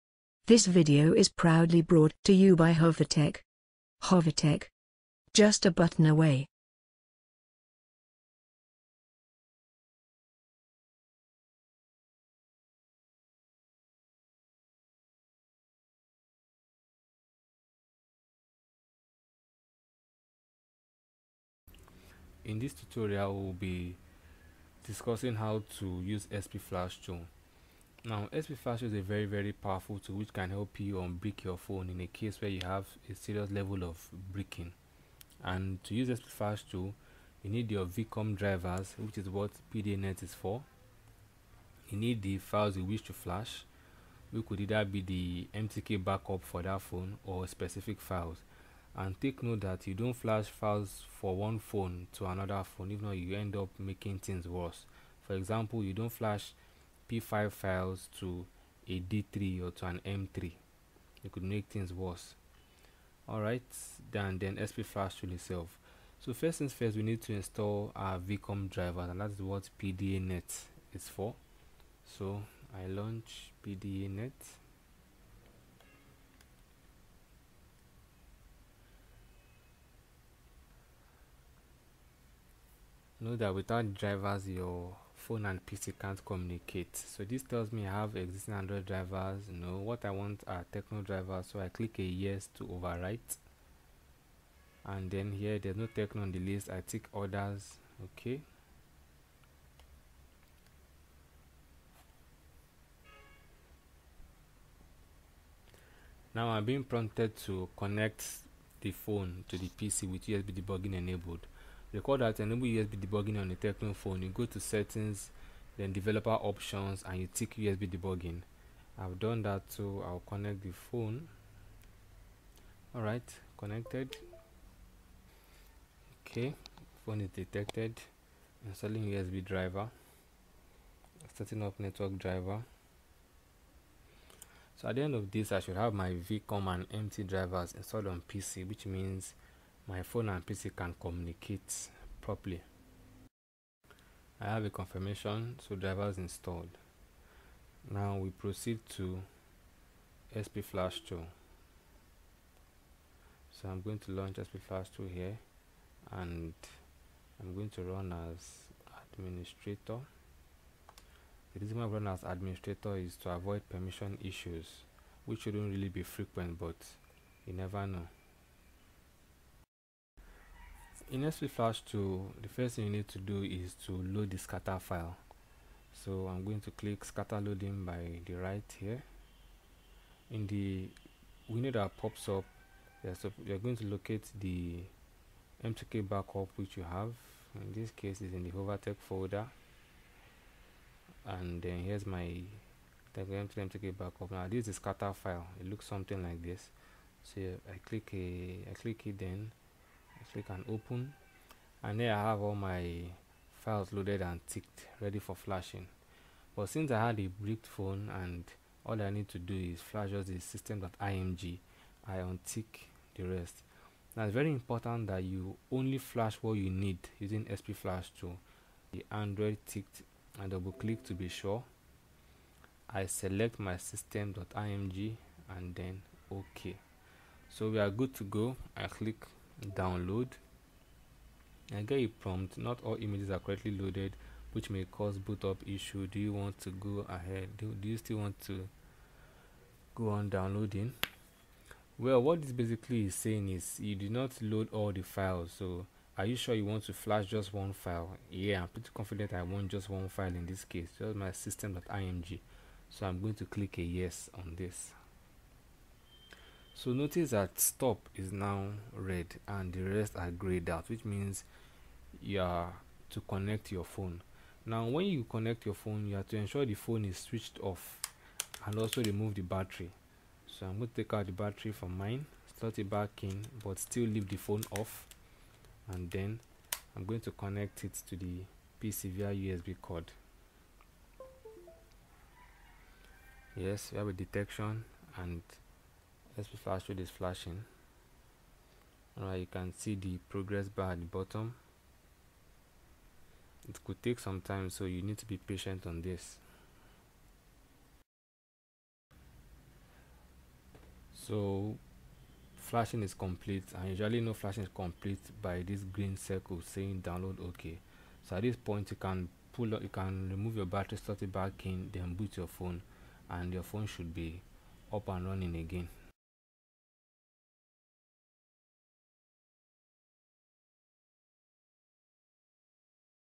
How To Use SP Flash Tool
You MUST install VCOM drivers as explained @ because the PdaNet drivers alone won't work on most models

00:22
In this tutorial, We'll be discussing how to use SP Flash tool. Now, SP Flash tool is a very powerful  tool which

00:30
can help you unbrick your phone in a case where you have a serious level of bricking.

00:34
To use SP Flash tool, you'll need your VCOM drivers which is what PdaNet is for. You'll need the files

00:42
you wish to flash, which could either be the MTK backup for that phone or specific files .

00:48
Take note that you don't flash files for one phone (model) to another phone (model) if not you'll end up

00:53
making things worse. For example, you don't flash (Tecno) P5 files to a D3 or an M3,

01:00
it could make things worse, and then SP Flash tool itself. First things first, we need to install our VCOM drivers

01:09
and that's what PdaNet is for (do a manual VCOM driver install if this fails). So I launch PdaNet

01:14
Note that without drivers, your phone and PC can't communicate (if this fails, install VCOM manually).

01:26
This tells me I have existing Android drivers so I click Yes to overwrite

01:33
Here, my phone brand isn't on the list so I tick Others, OK

01:39
Now, I'm being prompted to connect the phone to the PC with USB Debugging enabled.

01:51
To enable USB Debugging on an Android phone, you go to Settings - Developer Options

01:57
and you tick USB Debugging. I've done that so I'll connect the phone. Alright!

02:03
Connected! Phone is detected. Installing USB Driver. Setting up Network driver.

02:13
At the end of this, I should have my VCOM and MT drivers installed on PC

02:18
FOR NEWER MTK PHONES, DO A MANUAL VCOM DRIVER INSTALL. PDANET METHOD WON'T WORK

02:27
Now, we proceed to SP Flash tool. I'm going to run as Administrator.

02:40
The reason why we run as Administrator is to avoid permission issues which shouldn't be frequent.

02:48
In SP Flash tool, the first thing you need to do is to load the scatter file.

02:55
So I'm going to click Scatter-loading by the right here. In the Window that pops up, you locate the

03:04
MTK backup (firmware) which you have. In this case, its in the Hovatek folder.

03:09
Now, this is the scatter file, it looks something like this. So I highlight it then click Open

03:21
Here I have all my files loaded and ticked. Since I have a bricked phone and all I need to do is flash just the

03:30
system.img, I'll untick the rest. Its important that you only flash what you need in SP Flash tool.

03:38
I leave Android ticked. I double-click to be sure. I select my system.img and then click Open.

03:47
So we're good to go. I click Download. I get a prompt. Not all images are correctly loaded...

04:01
What this is saying is that you did not load all the files, are you sure you want to flash just one file?

04:08
I'm pretty confident I want just one file in this case, just my system.img. I'm going to click a Yes

04:16
Notice that Stop is now red and the rest are grayed out which means you're to connect your phone

04:24
Now, when you connect your phone, you're to ensure its switched off .

04:28
I'm going to take out the battery from mine and slot it back in (just power off if yours is inbuilt)

04:33
but still leave the phone off and then I'm going to connect it to the PC via USB cord.

04:40
[Alarm Clock] I have a detection and SP Flash tool is flashing. You can see the progress bar at the bottom.

04:52
It could take some time so you need to be patient. So Flashing is complete.

05:03
You'll usually know flashing is complete by this green circle saying Download OK

05:08
At this point, you can remove your battery and slot back in (if removable) then boot your phone

05:13
and your phone should be up and running again.